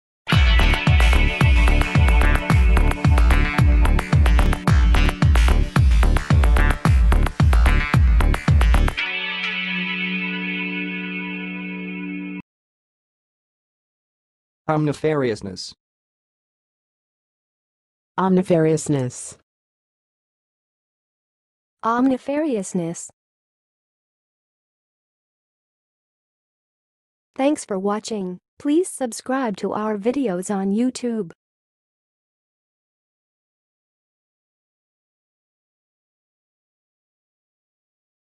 How to say "omnifariousness"! (High Quality Voices)
Watch in this video how to say and pronounce "omnifariousness"!